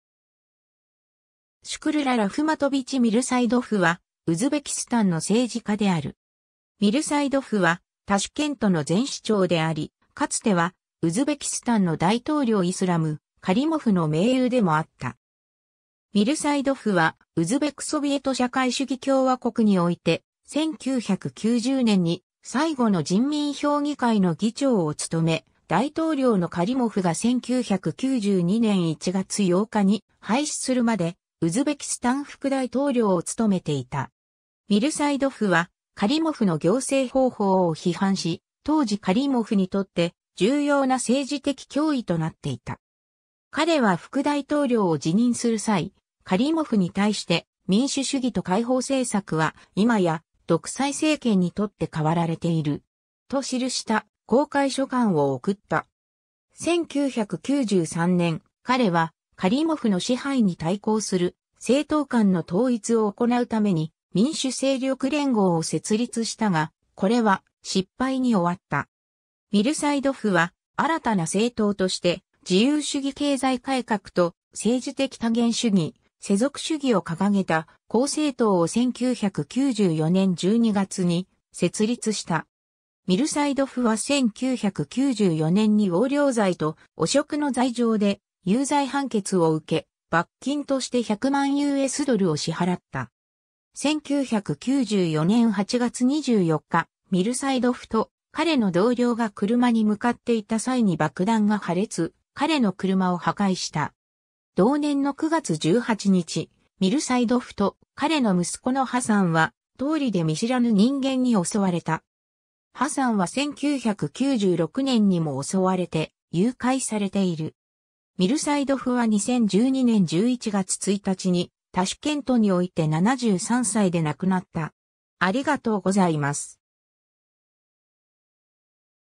シュクルッラ・ミルサイドフ
ウズベク・ソビエト社会主義共和国の首相
シュクルッラ・ラフマトヴィチ・ミルサイドフ（英語: Shukrullo Raxmatovich Mirsaidov、ウズベク語: Шукрулло Рахматович Мирсаидов、ロシア語: Шукрулла Рахметович Мирсаидов、1939年2月14日 - 2012年11月1日）は、ウズベキスタンの政治家である。
 ミルサイドフはタシュケントの前市長であり、かつてはウズベキスタンの大統領イスラム・カリモフの盟友でもあった。
ミルサイドフはウズベク・ソビエト社会主義共和国において、1990年に最後の人民評議会の議長を務め、大統領のカリモフが1992年1月8日に廃止するまでウズベキスタン副大統領を務めていた。
 ミルサイドフはカリモフの行政方法を批判し、当時カリモフにとって重要な政治的脅威となっていた。
彼は副大統領を辞任する際、カリモフに対して「民主主義と開放政策は今や独裁政権にとって変わられている。」と記した公開書簡を送った。
1993年、彼はカリモフの支配に対抗する政党間の統一を行うために民主勢力連合（Forum of Democratic Forces）を設立したが、これは失敗に終わった。
ミルサイドフは新たな政党として自由主義経済改革と政治的多元主義、世俗主義を掲げた公正党（Adolat）を1994年12月に設立した。
 ミルサイドフは1994年に横領罪と汚職の罪状で有罪判決を受け、罰金として100万USドルを支払った。
1994年8月24日、ミルサイドフと彼の同僚が車に向かっていた際に爆弾が破裂、彼の車を破壊した。
同年の9月18日、ミルサイドフと彼の息子のハサンは通りで見知らぬ人間に襲われた。
ハサンは1996年にも襲われて誘拐されている。
 ミルサイドフは2012年11月1日にタシュケントにおいて73歳で亡くなった。